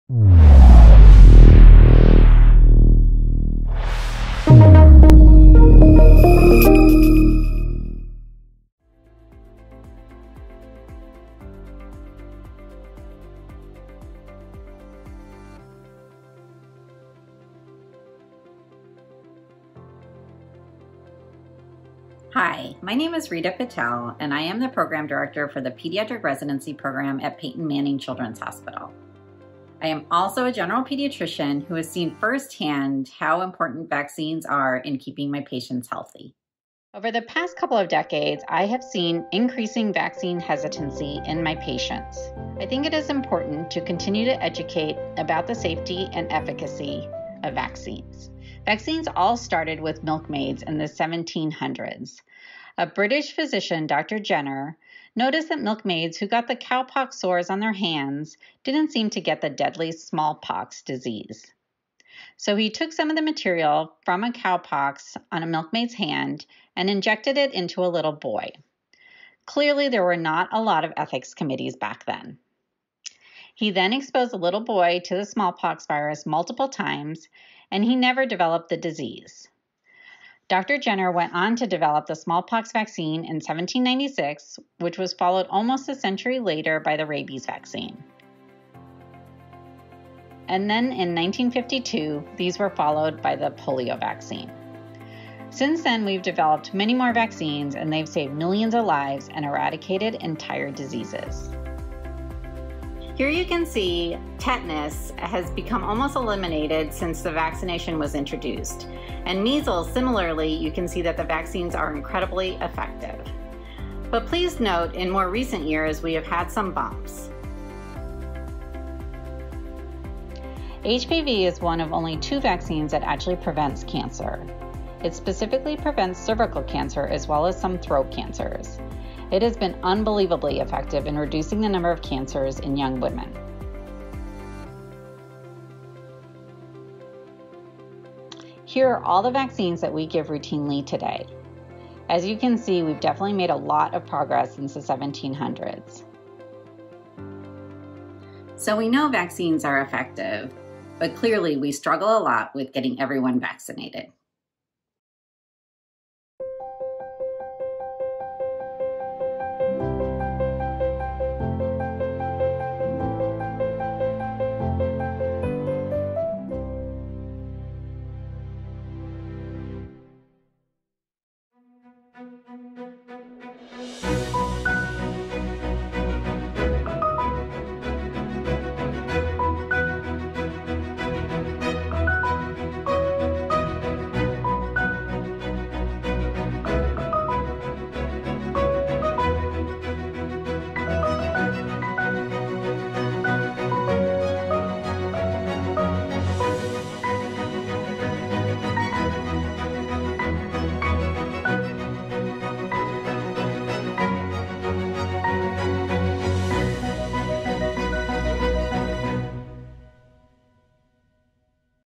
"La Historia de las Vacunas" con subtítulos en español.
esde las primeras vacunas contra la viruela en el siglo XVIII, se ha demostrado que las vacunas a lo largo de las décadas son seguras y eficaces.